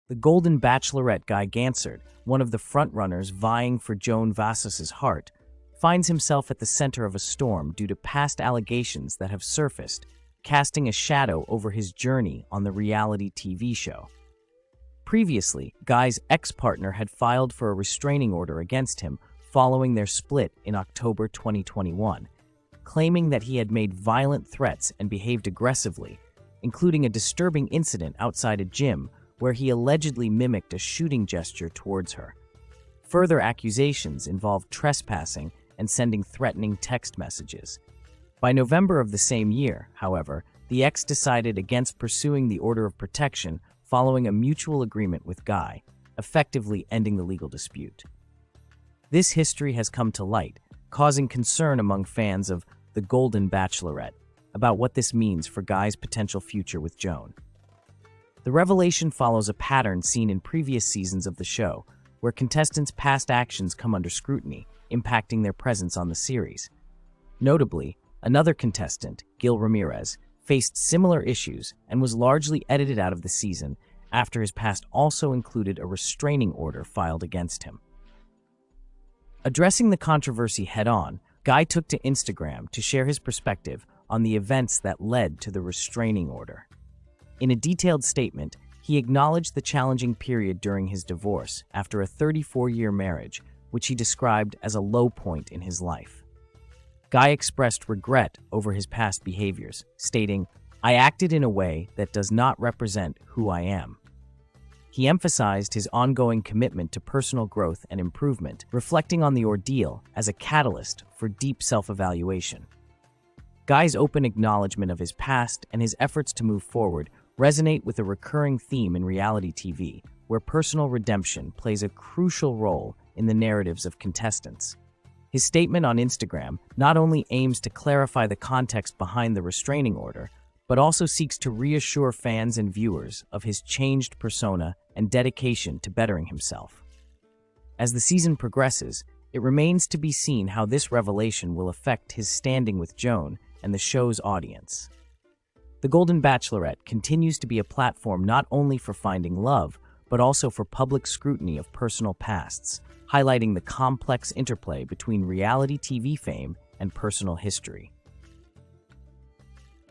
Golden Bachelorette: Guy Gansert Finally Speaks About His Restraining Order
The Golden Bachelorette Guy Gansert, one of the frontrunners vying for Joan Vassos's heart, finds himself at the center of a storm due to past allegations that have surfaced, casting a shadow over his journey on the reality TV show.